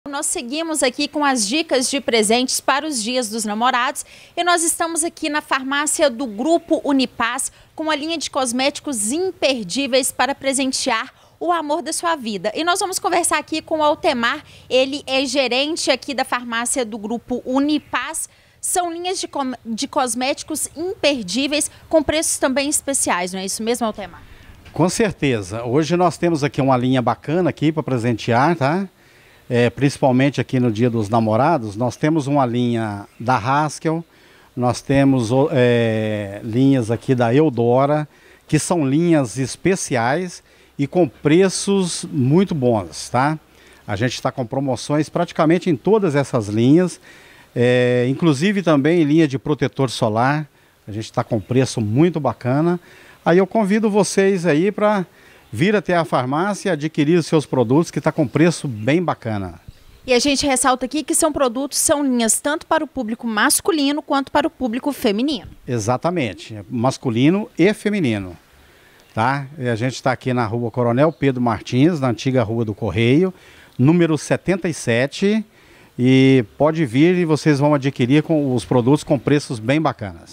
Dia dos Namorados: Farmácia Uni e Paz está com linha de cosméticos especiais
Dia dos Namorados: Farmácia Uni e Paz está com linha de cosméticos especiais para a data mais romântica do ano

12 de junho, Dia dos Namorados, uma data marcada por lindas declarações de amor, mensagens de carinho e troca de presente entre os casais apaixonados. Em clima de romantismo, a Farmácia Uni e Paz está com linhas de cosméticos incríveis para presentear aquele que você ama.

Linha completa da Eudora, Haskell e protetor solar. Produtos de qualidade e preços especiais. A Farmácia Uni e Paz está situada na rua Coronel Pedro Martins, 77, na conhecida “Rua do Correio”, em Caratinga.